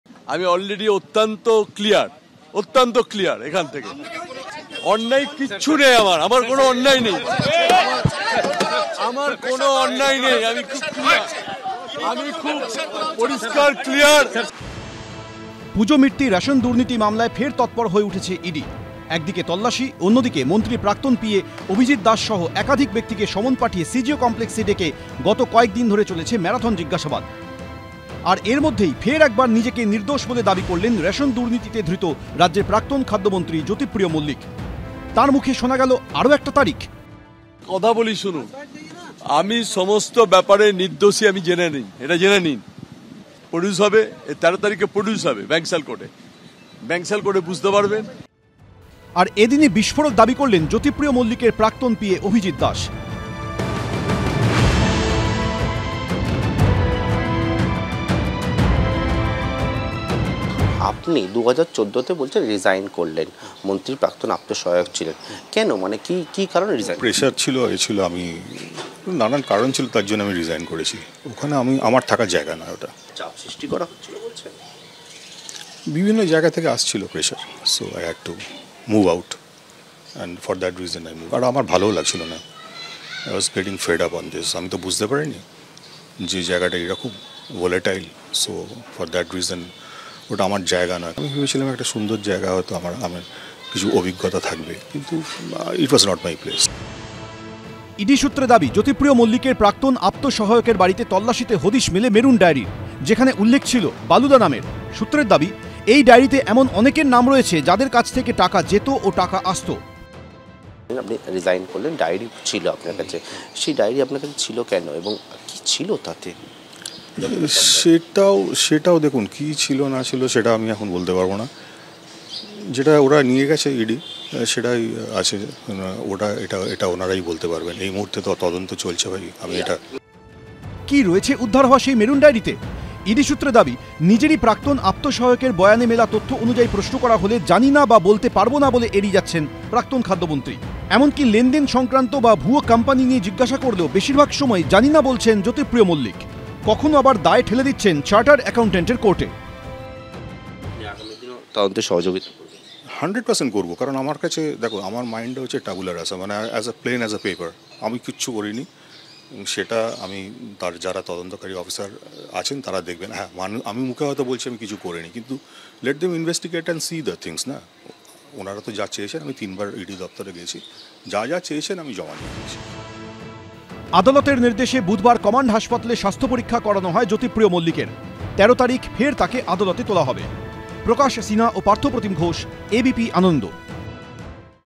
Ration Scam : আজ ফের নিজেকে নির্দোষ দাবি করলেন রেশন দুর্নীতিতে ধৃত মন্ত্রী জ্যোতিপ্রিয় মল্লিক
ABP Ananda Live :আজ ফের নিজেকে নির্দোষ দাবি করলেন রেশন দুর্নীতিতে ধৃত মন্ত্রী জ্যোতিপ্রিয় মল্লিক